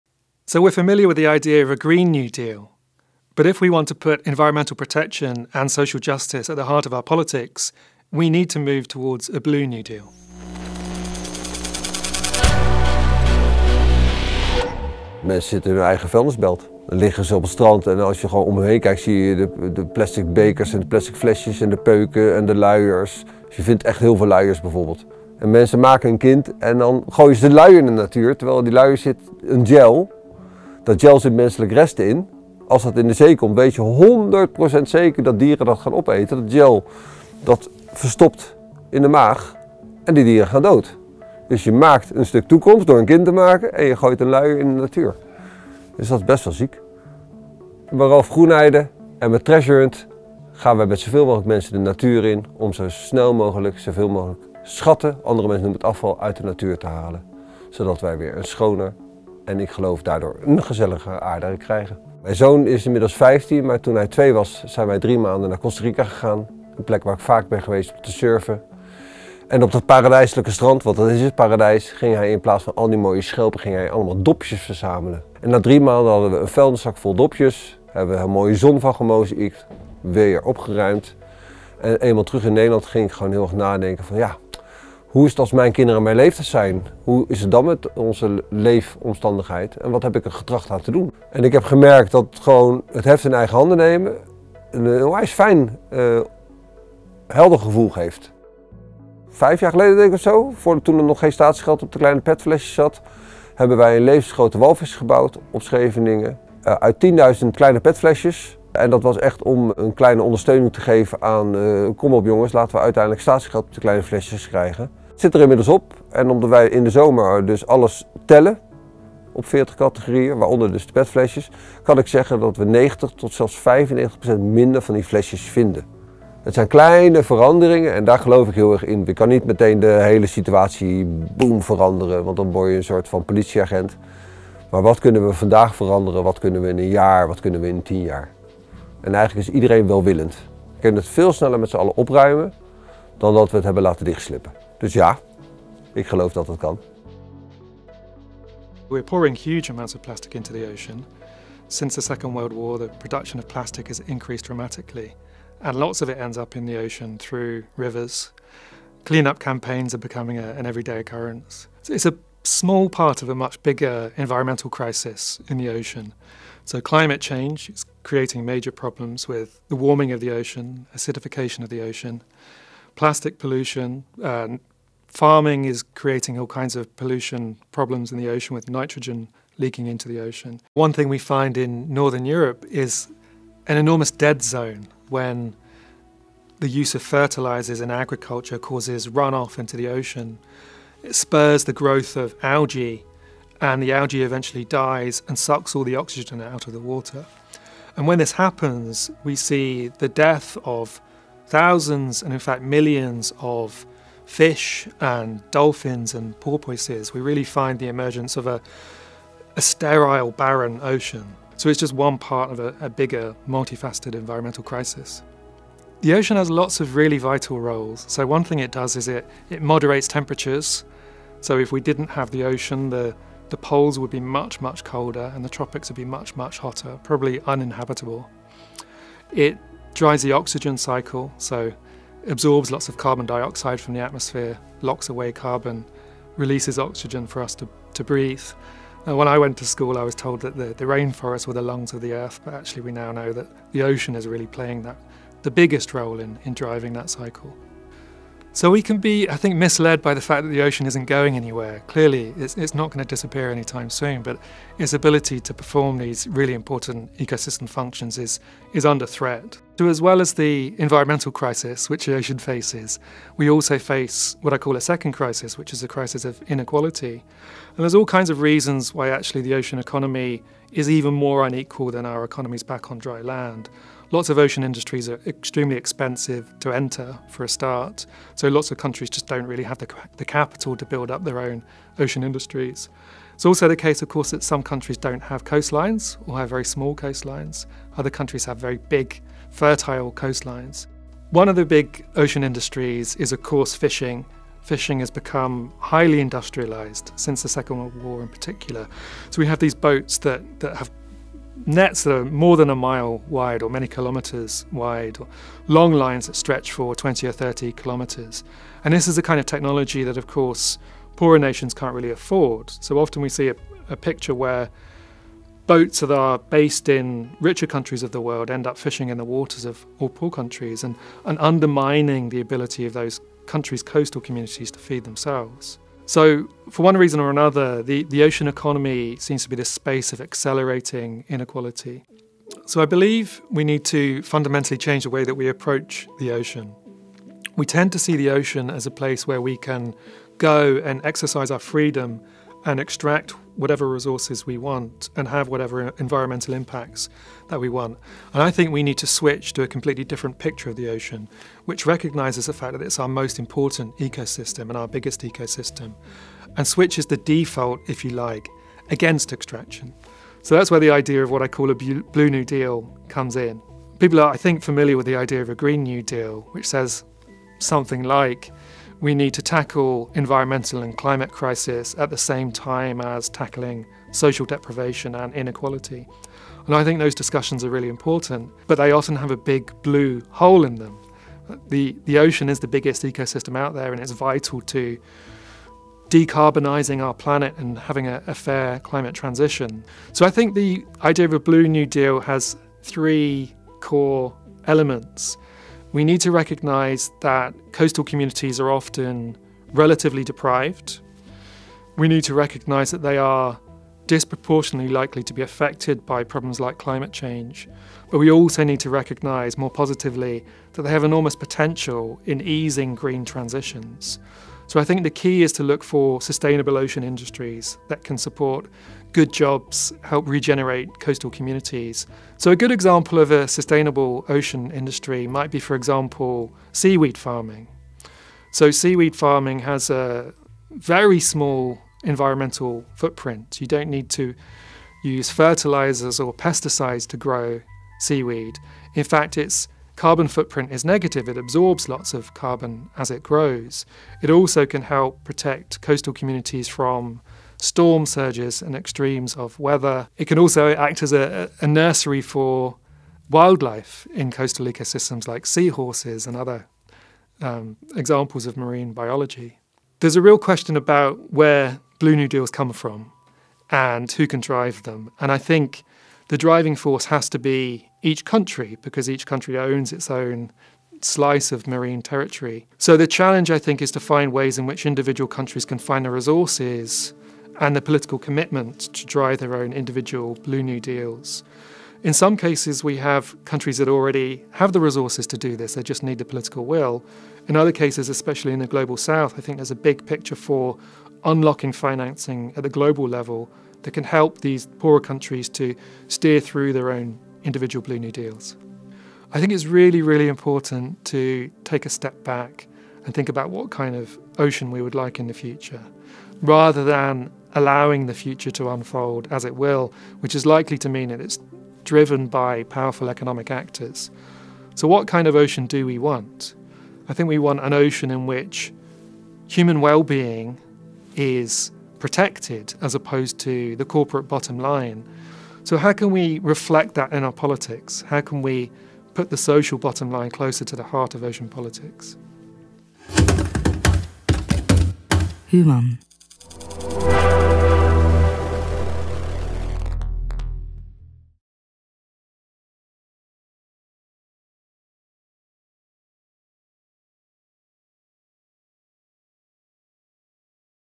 Brainwash Bits: Why we need a Blue New Deal | Chris Armstrong
De opwarming en verzuring van oceanen, enorme hoeveelheden plastic soep en het ontstaan van dead zones, grote gebieden waar geen (onderwater)leven meer mogelijk is. Dat het niet goed gaat met de oceanen en zeeën is wel duidelijk. En dat terwijl ze een sleutelrol spelen in het voortbestaan van de planeet.

Volgens hoogleraar politieke theorie Chris Armstrong schieten overheden tekort in het bestrijden van deze crises. Hij pleit voor een Blue New Deal: afspraken die de oceanen weer gezond moeten maken en zorgen voor een eerlijkere verdeling van economische hulpbronnen uit die oceanen en zeeën.

Kijkwijzer: AL